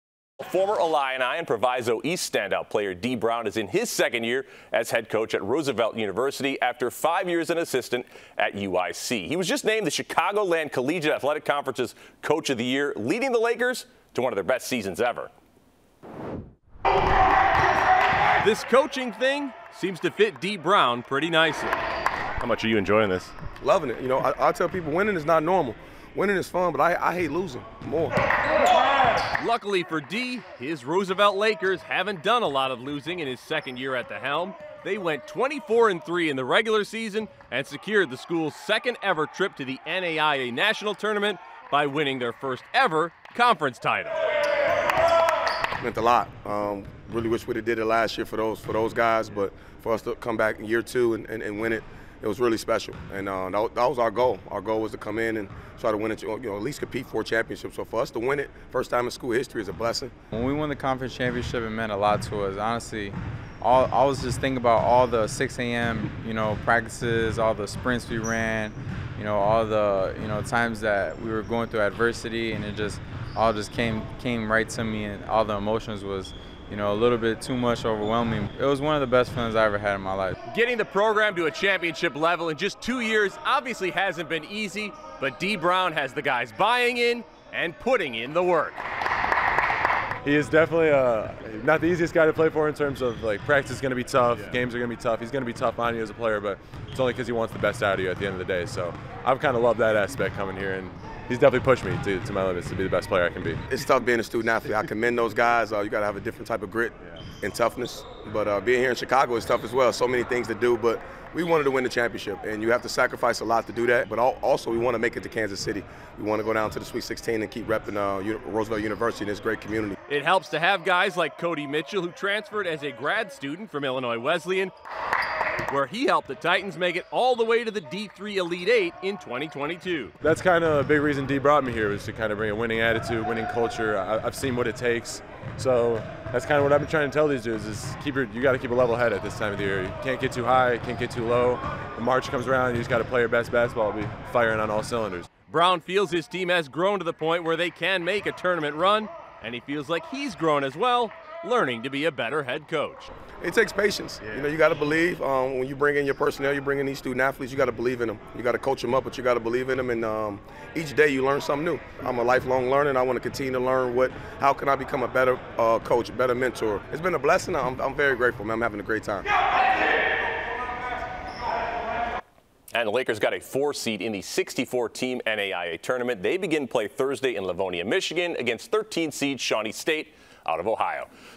Roosevelt men's basketball thriving under Coach Dee Brown
Brown was just named the Chicagoland Collegiate Athletic Conference coach of the year.